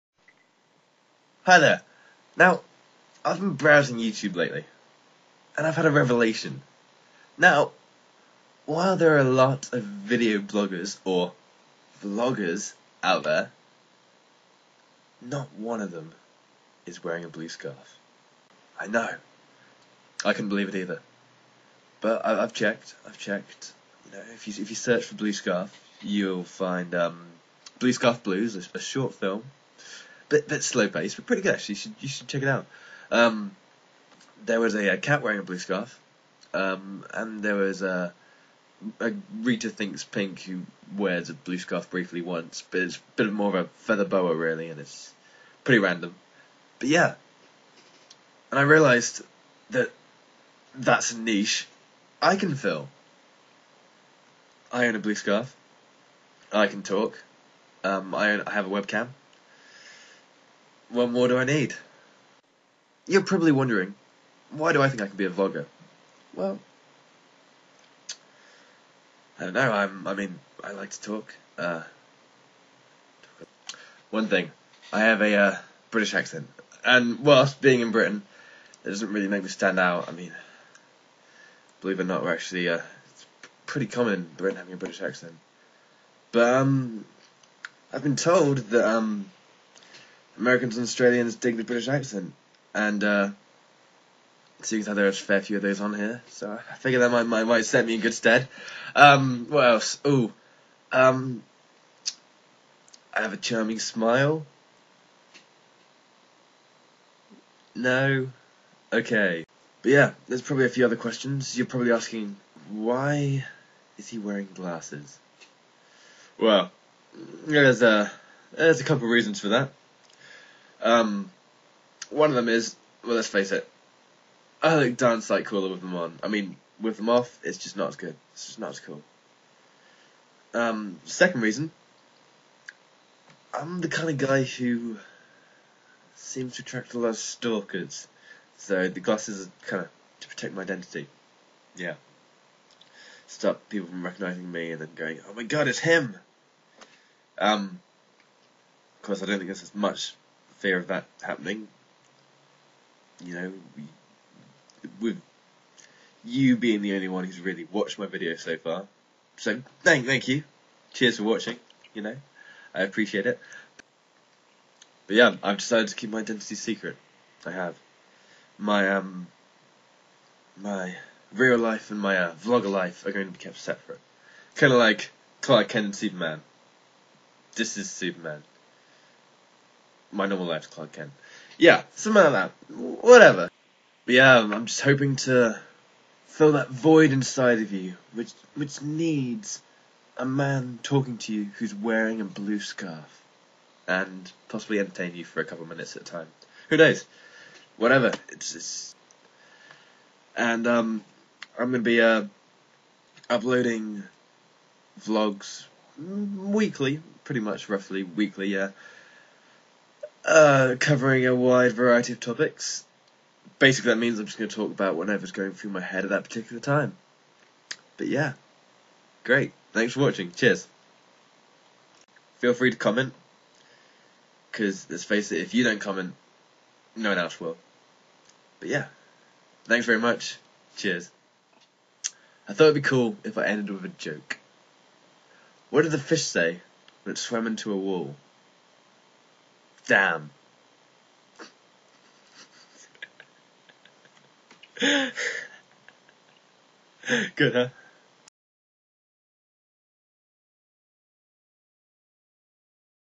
Introduction
1 man. 1 blue scarf . 1 laptop. 1 webcam. 1 free video editing program. 5 fruit and veg a day. 2 turtle doves. 1 addiction with creating number lists.
And. 1 introductory video